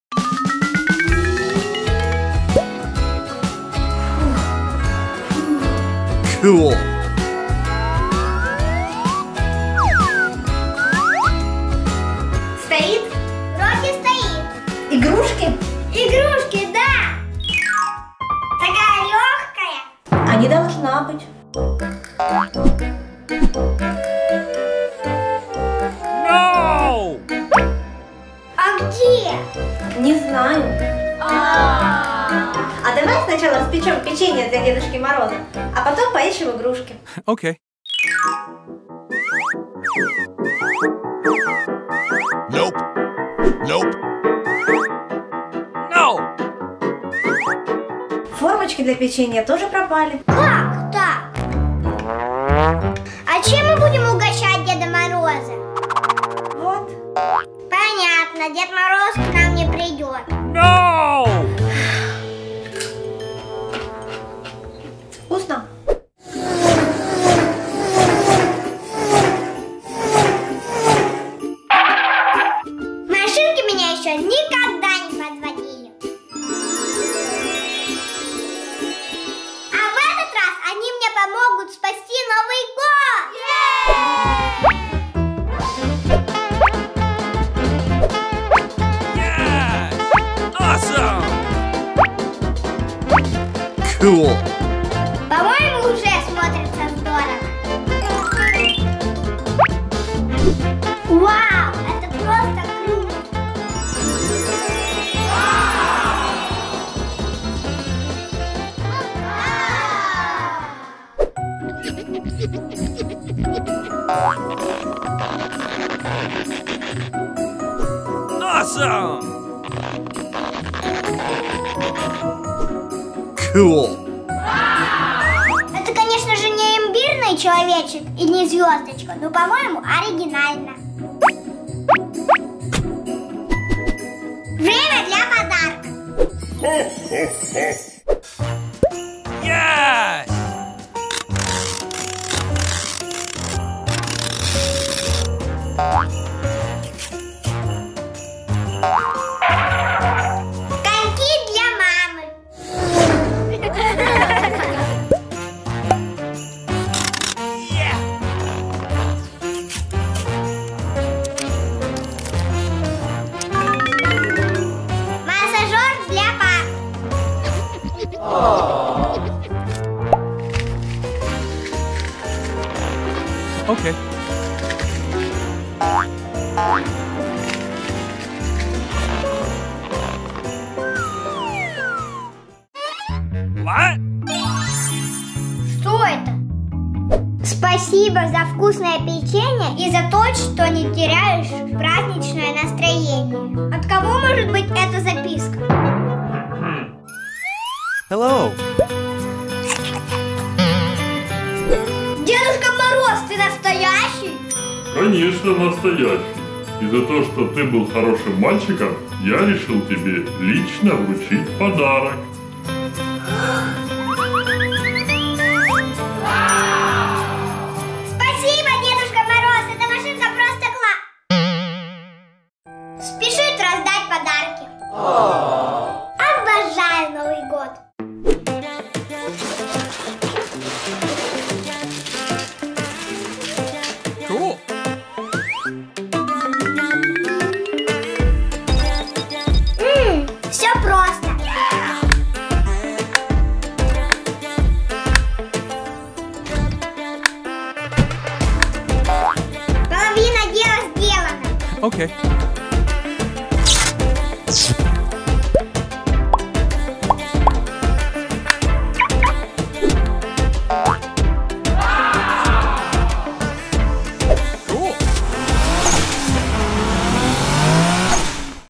Cars help Mark to get ready for the New Year
Mark is getting ready for the New Year and cars help him.